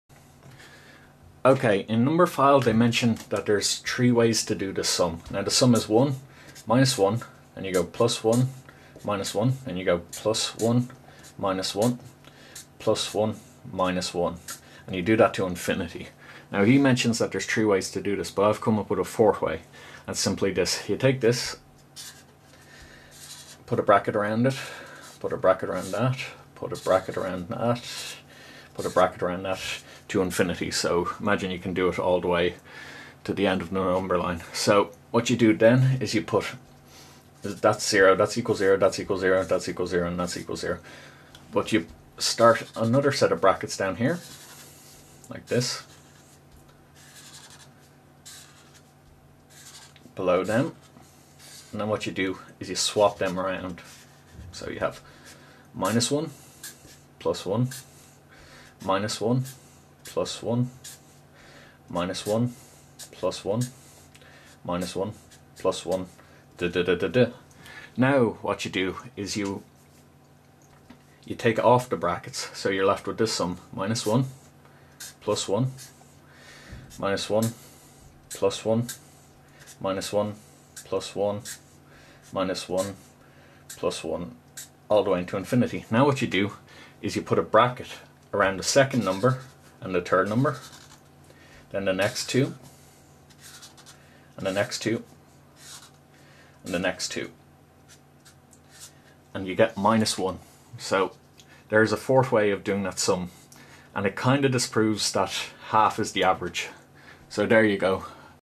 Numberphile infinite series new sum.
In this video I show a new way to put together the infinite sequence of 1-1+1-1+1-1.